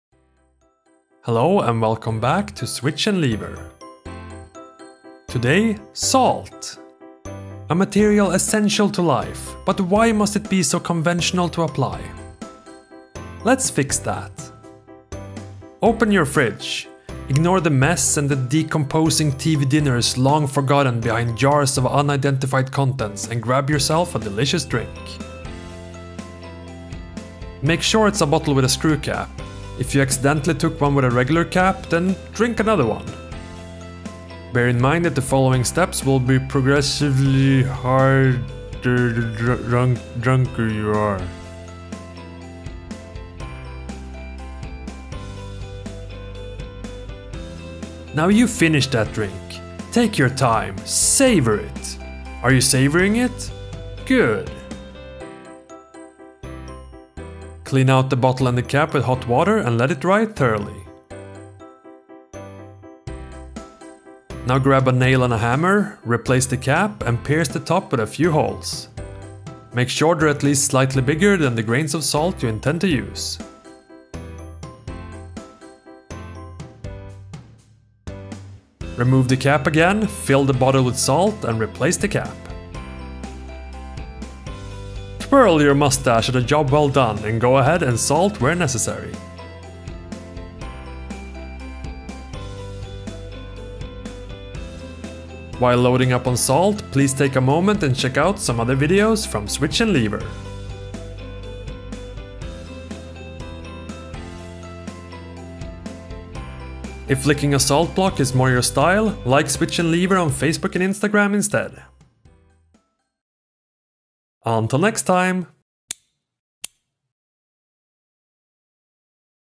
Hipster Salt Shaker
Who doesn't need salt? You need salt, I need salt, even hipsters need salt! Why be a conventional salt hipster? I was into salt before it was cool! Make a hipster salt shaker and impress all your friends!

Images:
Rock salt crystals by włodi